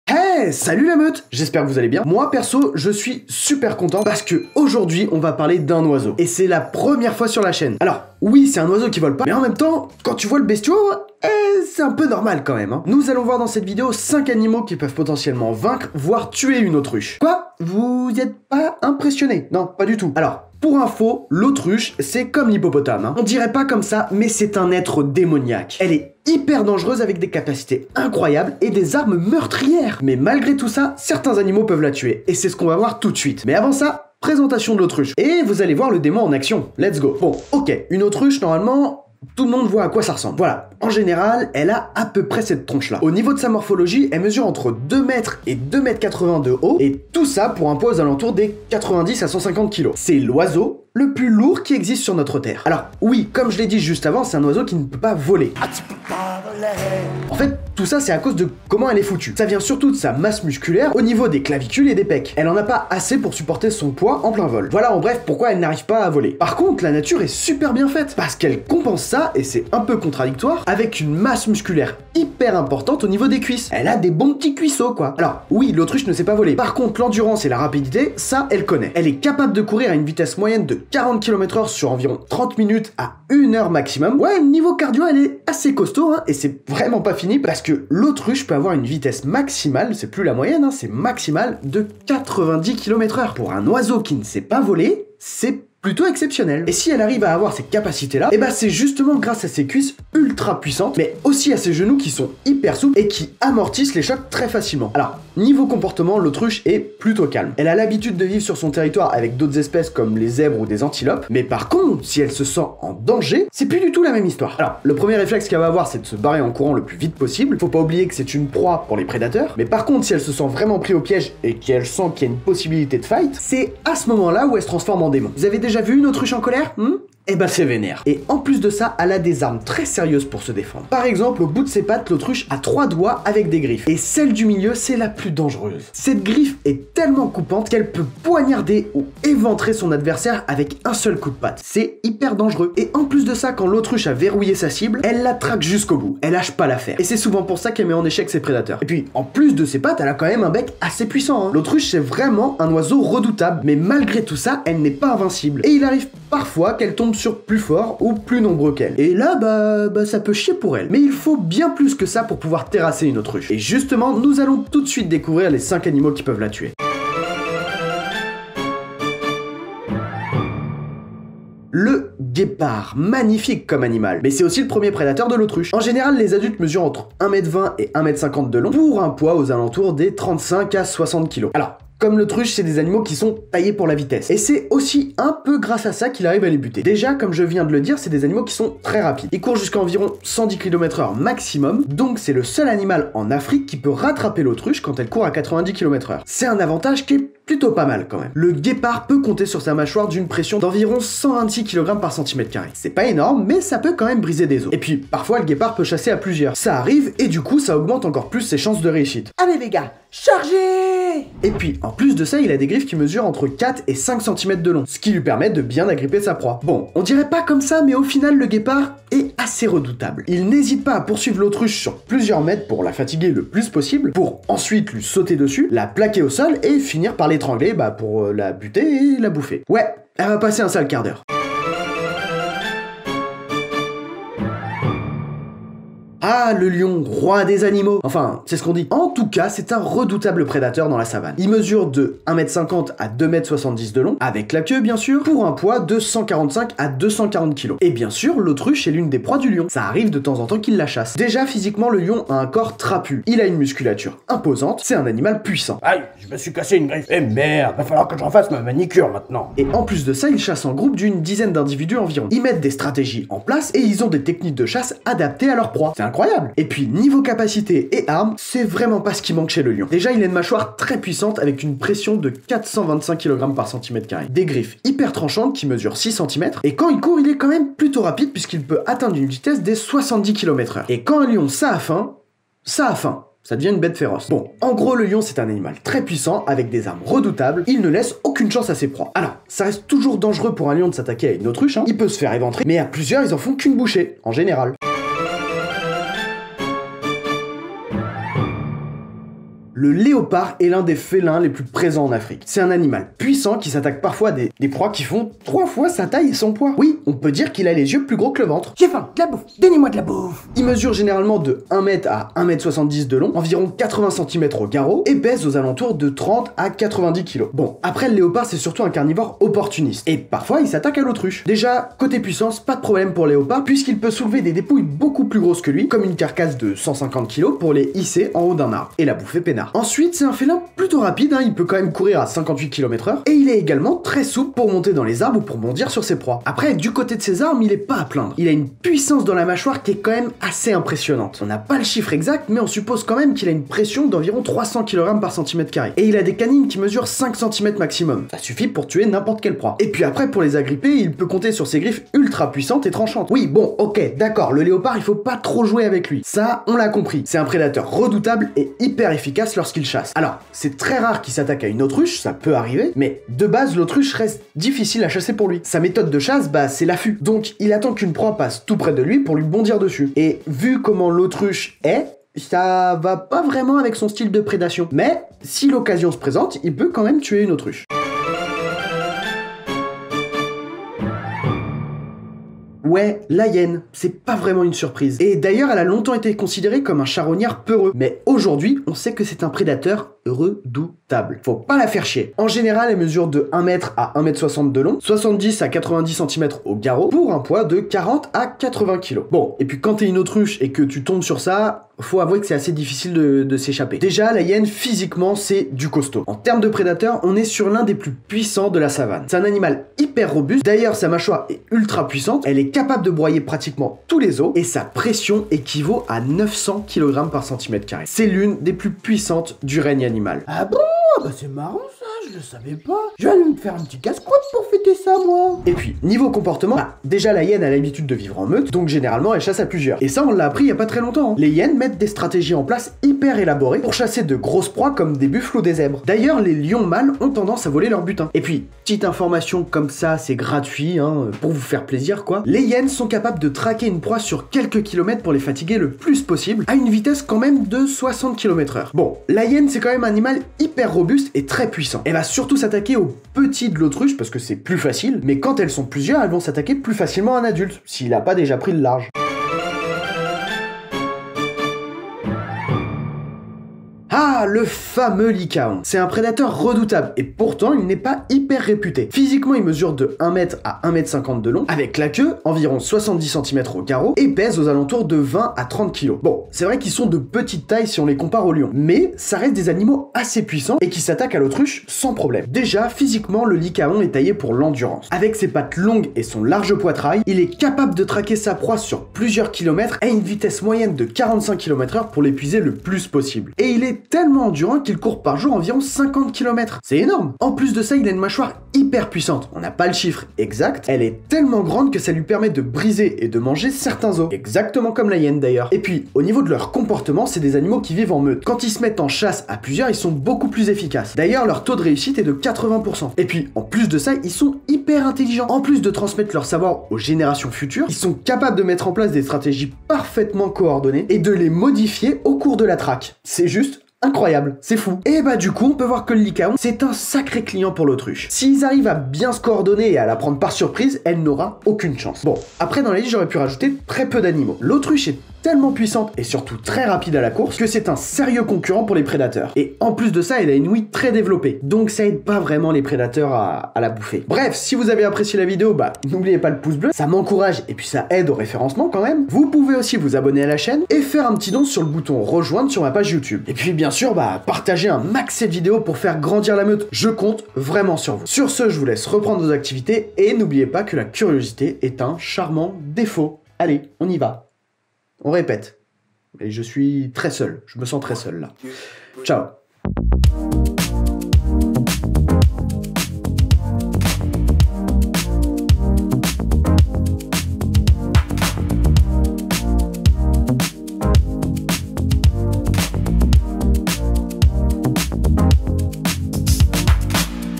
5 ANIMAUX qui pourraient VAINCRE une AUTRUCHE
L'Autruche est très dangereuse, mais elle n’est pas invincible… Et il arrive parfois qu’elle tombe sur plus fort qu'elle. Voici 5 ANIMAUX QUI POURRAIENT VAINCRE UNE AUTRUCHE.

Tu veux aider la meute et bénéficier d'avantages exclusifs ?

Sources :
Présentation de l'Autruche :